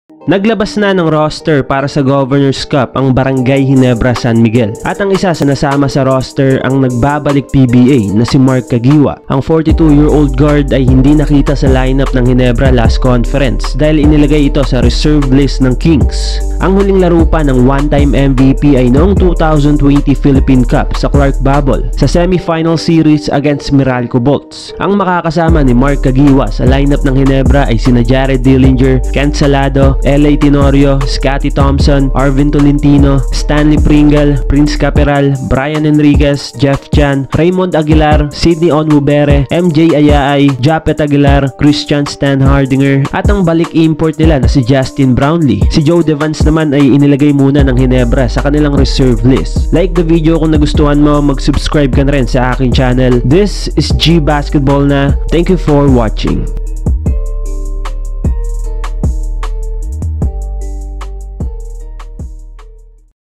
Mark Caguioa babalik na sa PBA! | Brgy.Ginebra San Miguel Official Lineup
Ibinalik na sa lineup ng brgy.ginebra ang 1 time MVP na si Mark Caguioa.
Last conference hindi nakasama si Mark sa lineup ng Ginebra at inilagay lang ito sa reserve list.

Credits to ONE SPORTS, PBA, SPIN. PH and other respectful owner/s of the images and videos that has been used in this video.